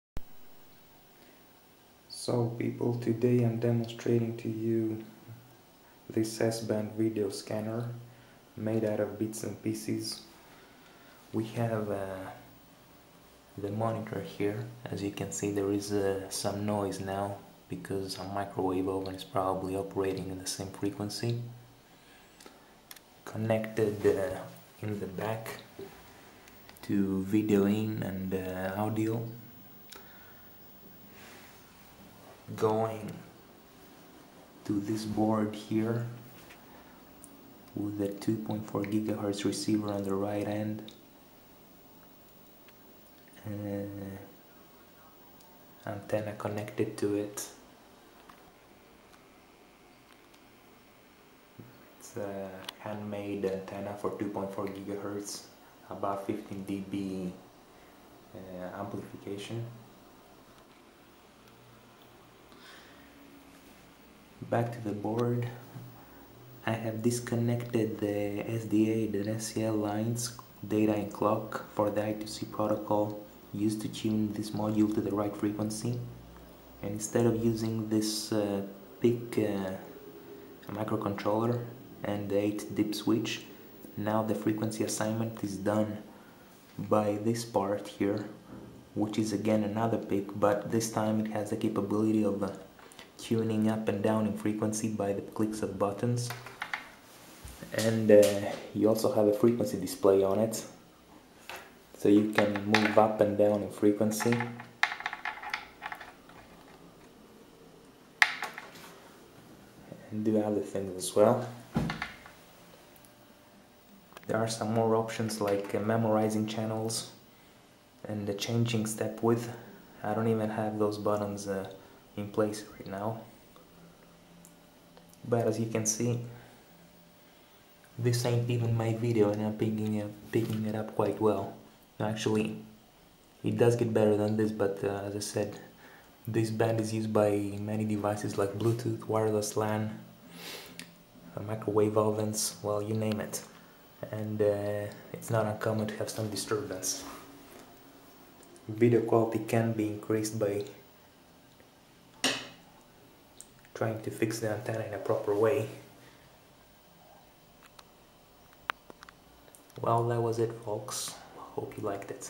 2.4 Ghz video scanner
An overview of the bits and pieces I used to setup a 2.4 GHz video scanner.
Tuning range is now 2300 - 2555 MHz in steps from 125 kHz to 10 MHz.

- a 5 " monitor with external video in selector switch
- 2.4 GHz rx module and board tunable with I2C.
- a board with a PIC micro for sending the appropriate I2C commands.
- pair of RCA cables to connect Rx-board and monitor
- a directional antenna for 2.4 GHz